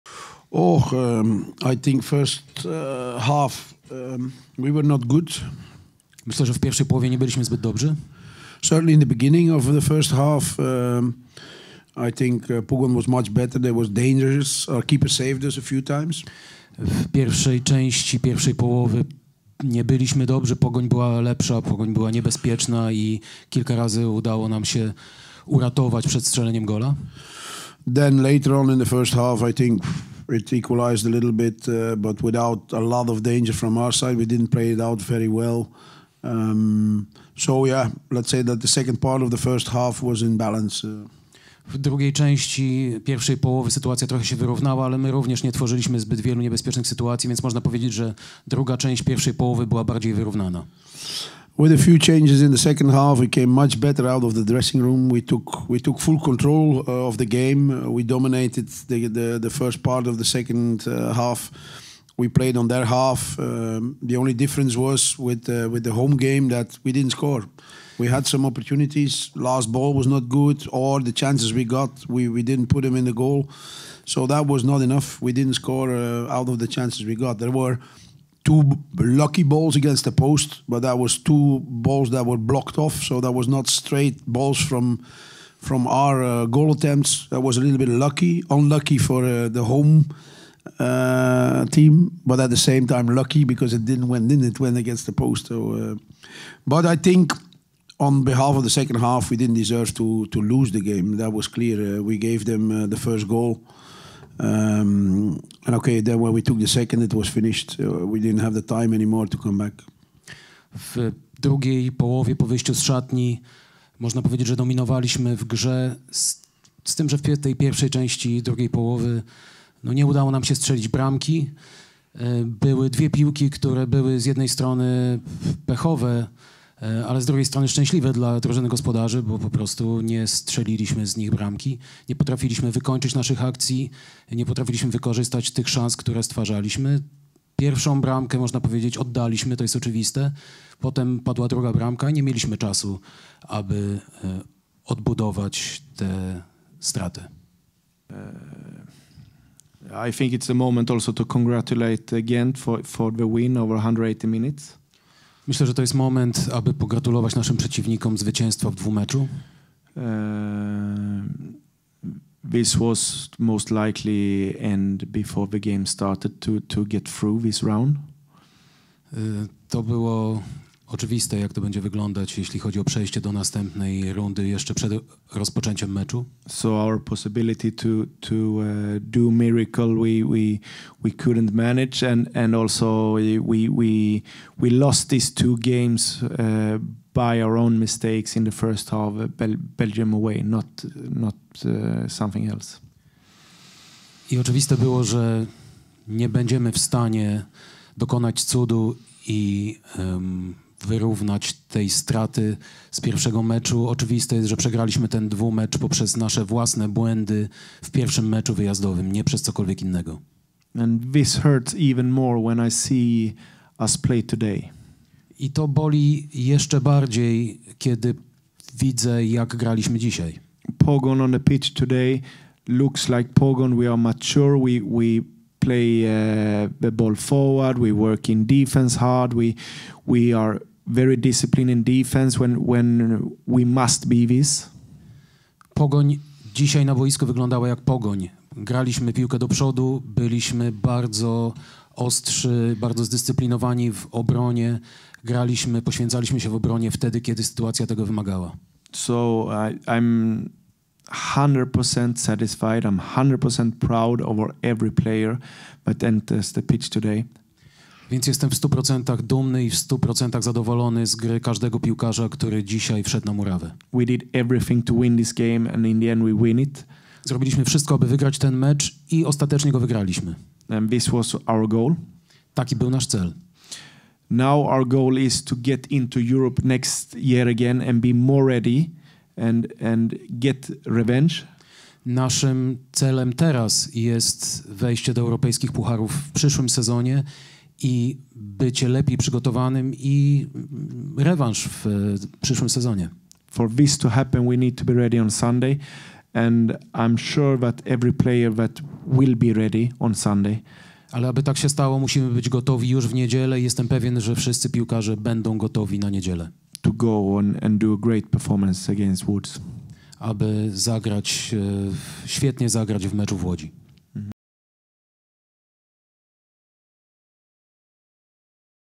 KONFERENCJA | Hein Vanhaezebrouck i Jens Gustafsson po
Trenerzy Radomiaka i Pogoni Szczecin na pomeczowej konferencji prasowej.
UEFA Europa Conference League, III runda eliminacji, rewanż
Pogoń Szczecin - KAA Gent 2:1 (0:0)
Gole:
1:0 - Efthymios Koulouris 79’
2:0 - Efthymios Koulouris 86’
2:1 - Hugo Cuypers 90’